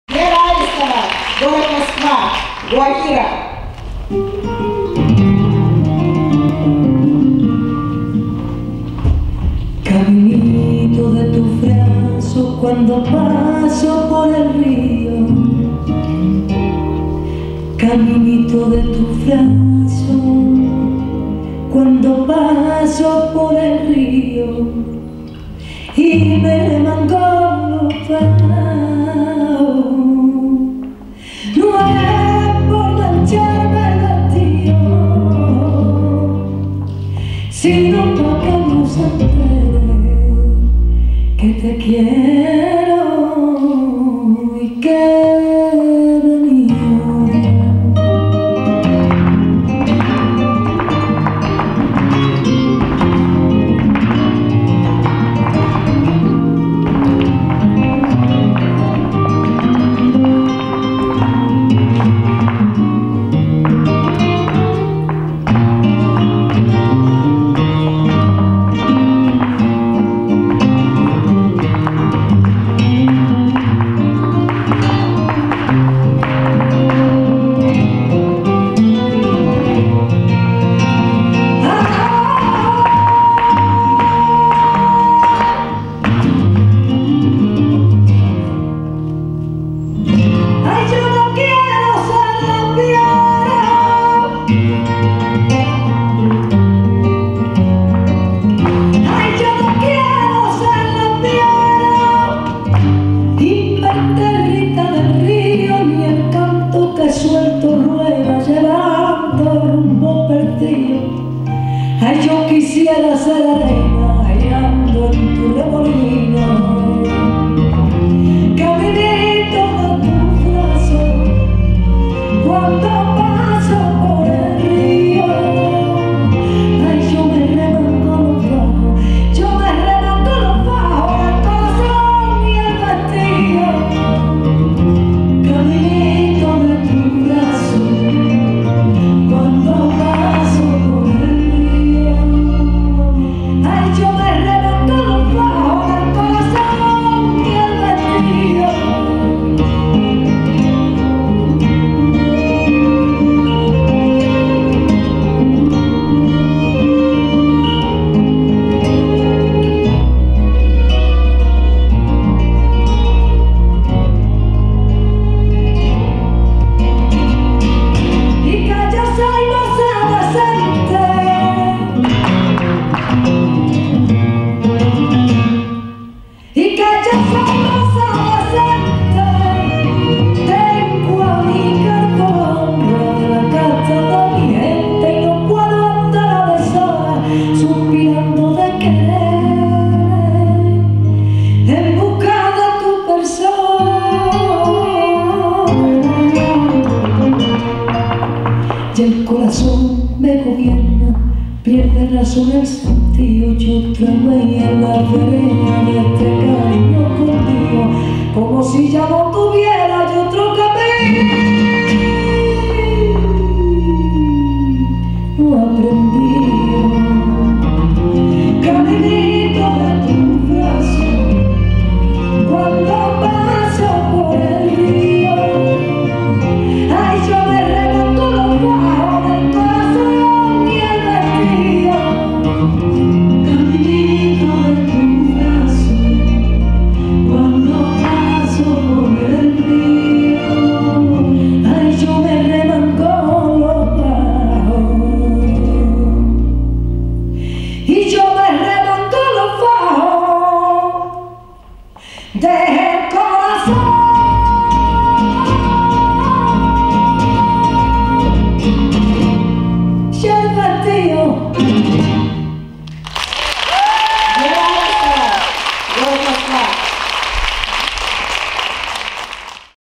* Palomas de Santiago * Гуахира ( г. Москва ) * ФЛАМЕНКО над ВОЛГОЙ - 2021 *
VI - Всероссийский фестиваль. Тольяттинская филармония - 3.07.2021 г.